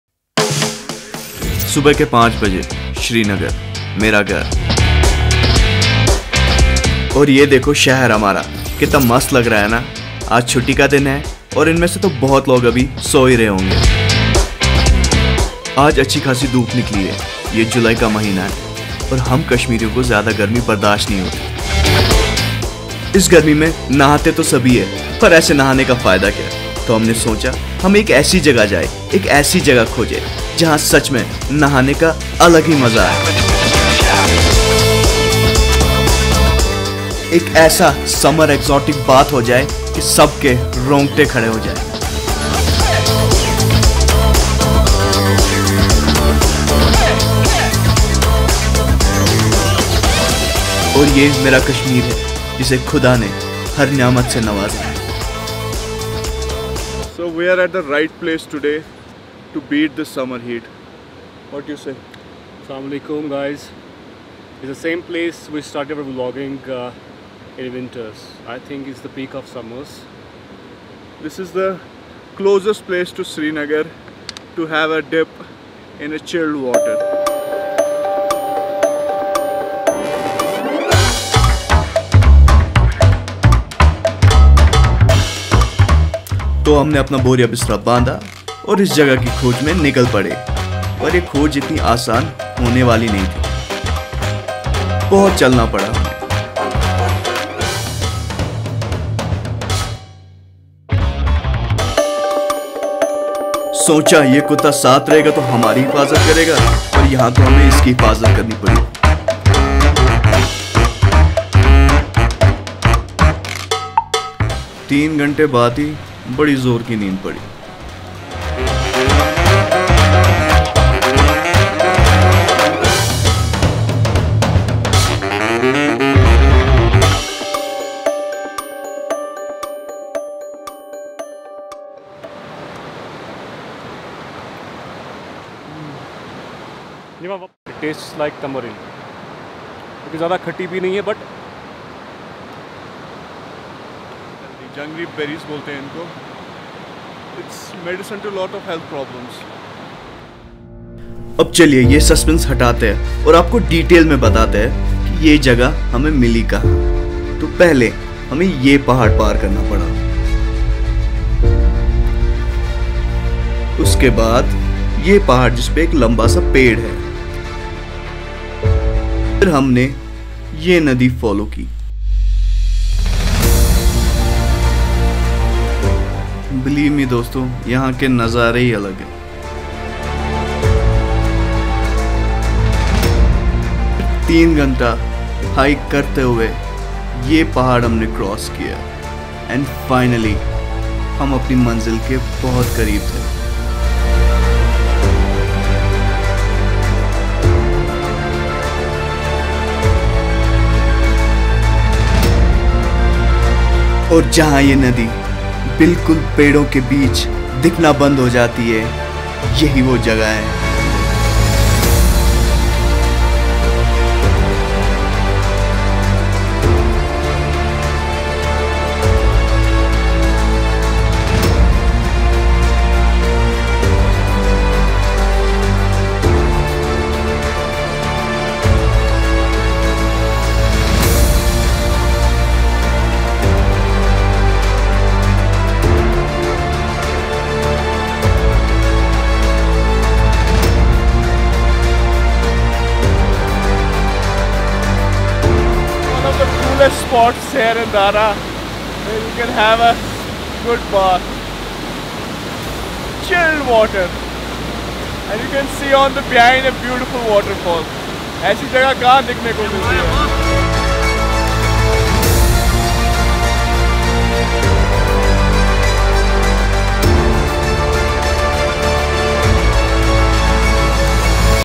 Trekking into the wild-Jammu and Kashmir
This vlog is about finding the perfect spot to to beat the summer heat. Our quest took us far to the dara theed area to find the most beautiful location to take a dip in the cold soul soothing water in between the beautiful and wild nature